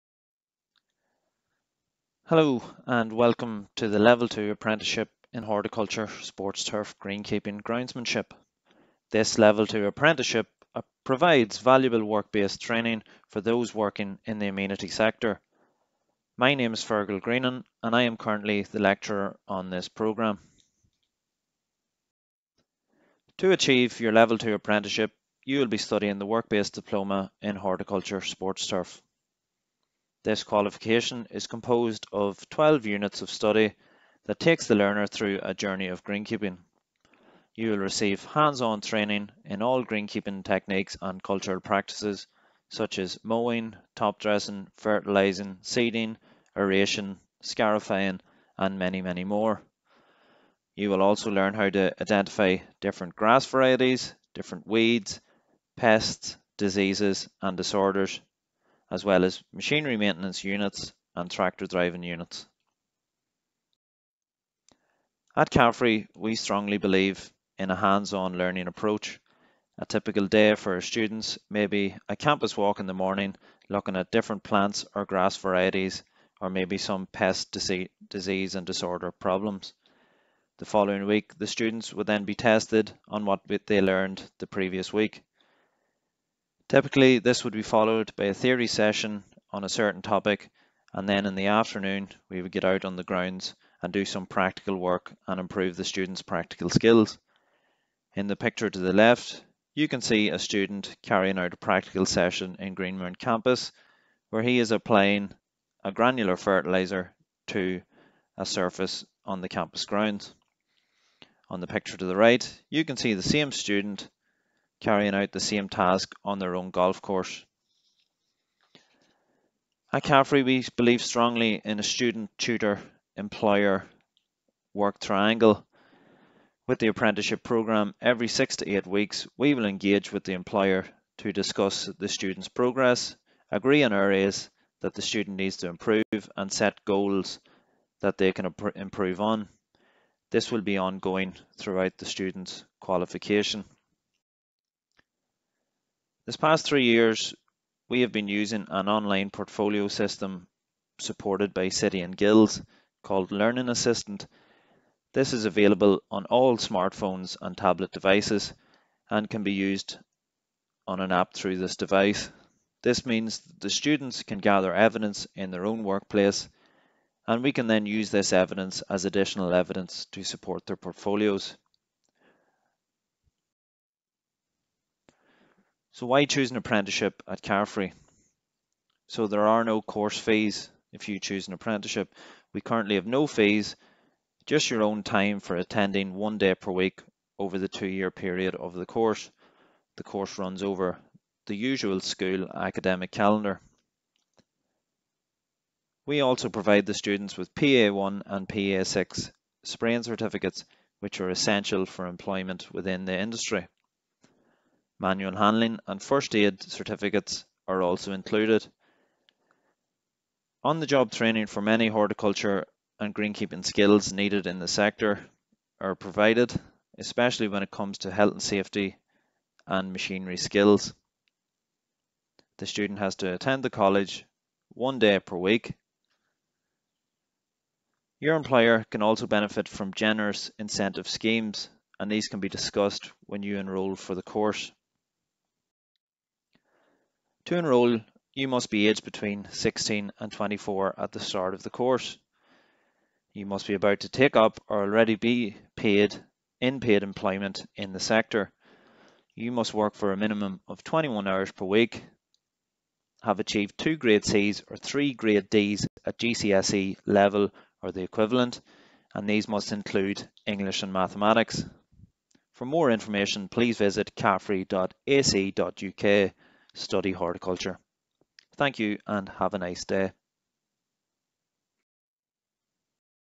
Horticulture Apprenticeship Sportsturf - Greenkeeper
Keep your eye on the ball. Study for a qualification in Sportsturf at CAFRE Greenmount Campus. A Level 2 Apprenticeship course offers you the chance to earn as you learn with CAFRE.